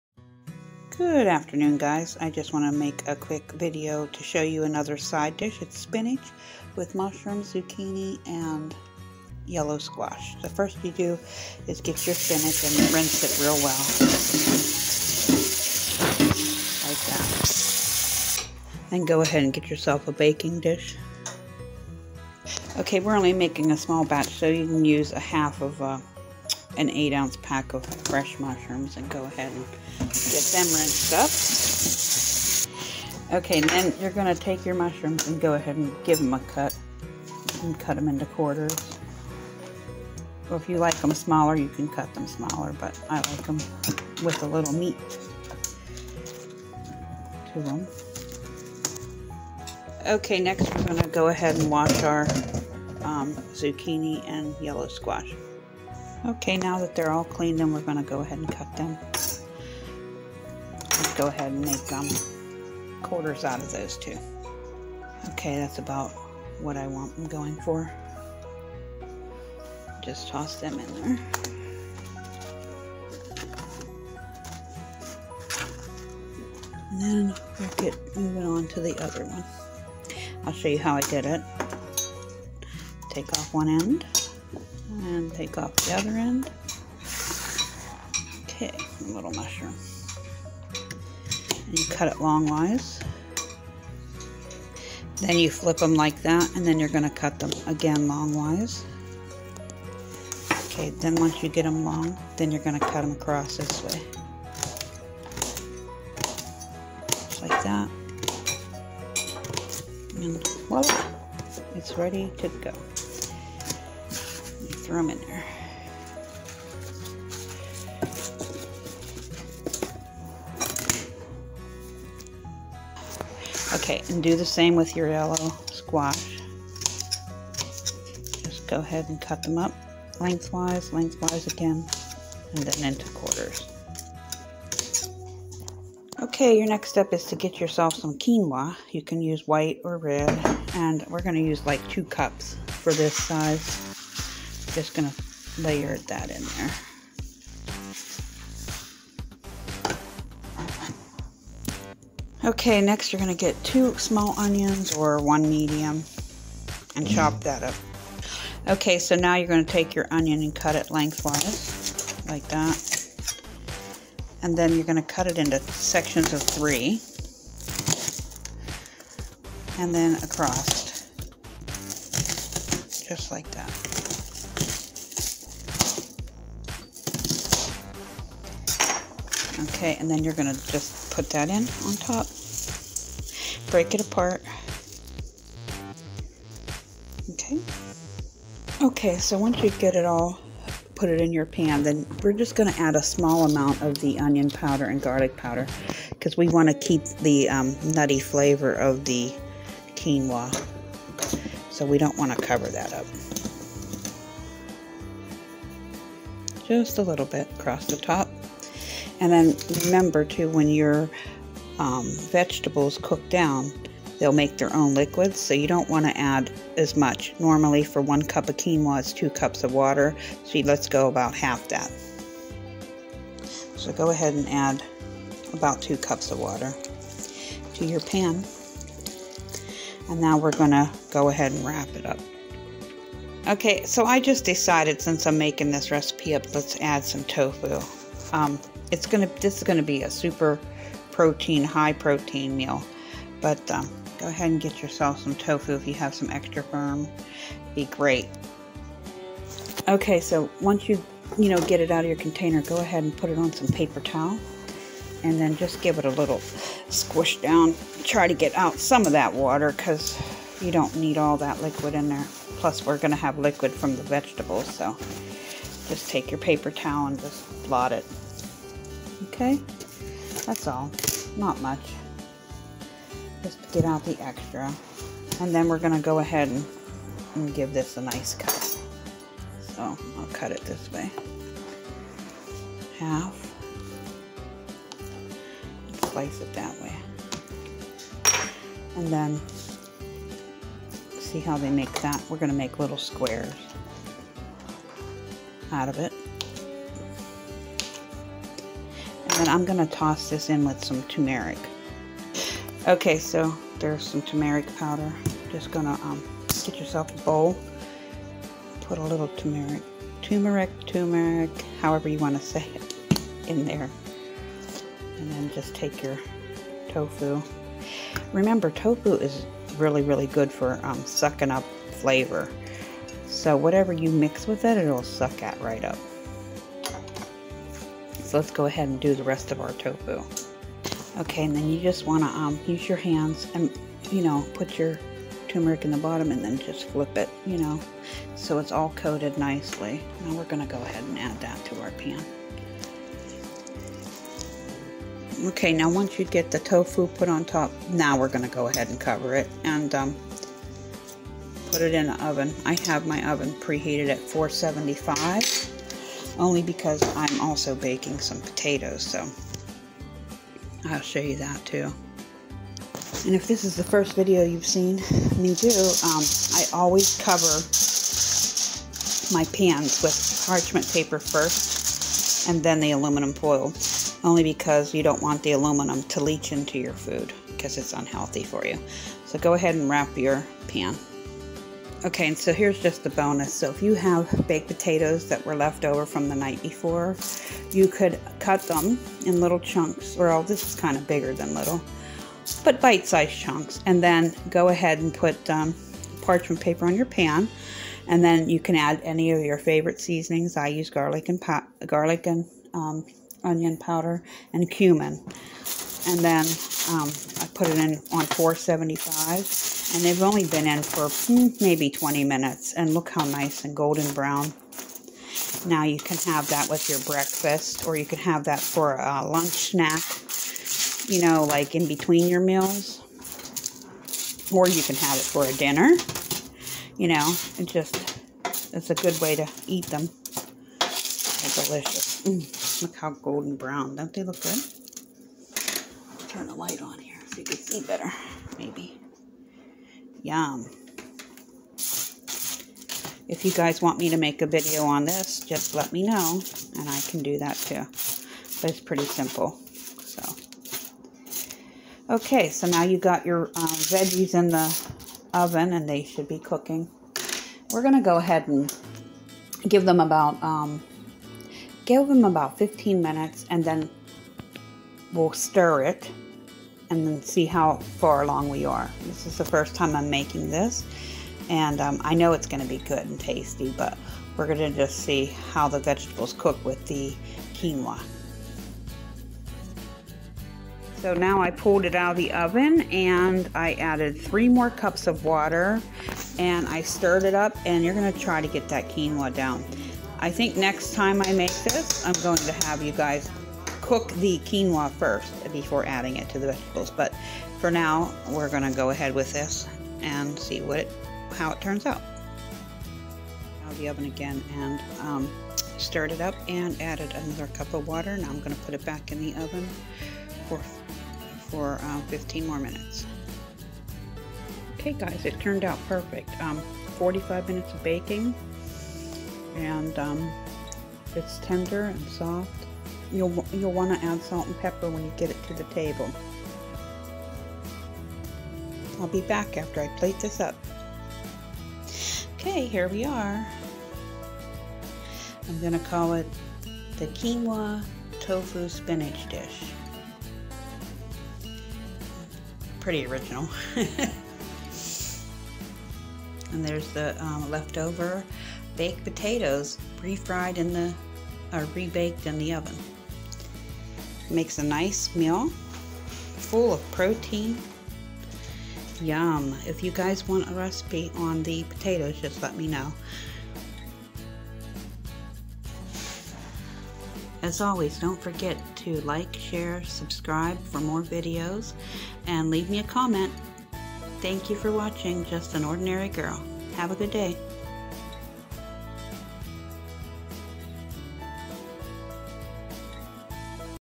Quinoa Spinach Tofu (QTS DISH) Original Vegan Recipe - Just An Ordinary Girl
INGREDIENTS:

6 cups of water
2 cups of quinoa
1/2 package of mushrooms (cubed)
2 small onions (quartered, then chopped)
2 small zucchini (cubed)
1 small yellow squash (cubed)
1 package extra firm tofu (cubed)
1/2 Large Bag Spinach
Garlic Powder
Onion Powder
Turmeric
Salt
Pepper

THINGS YOU'LL NEED:

One 13x9 Glass Baking Dish
Parchment Paper
Aluminum Foil

DIRECTIONS:

1. Wash spinach (add to pan)
2. Cut veggies (add to pan)
3. Add 2 cups Quinoa (add to pan)

You may cook your quinoa ahead of time, but you don't need to.

4. Add Garlic and Onion Powder generously over top of veggies in the pan
5. Add 2 Cups of Water to start with in your pan
6. Mix turmeric with tofu until coated and put on top of veggies
7. Cover and bake for .

15 minutes - stir add 3 cups of water
15 minutes - stir add 1 cup of water
15 minutes more, then done
Add salt and pepper to taste on your plate

Bake at 475 degrees Fahrenheit for 45 minutes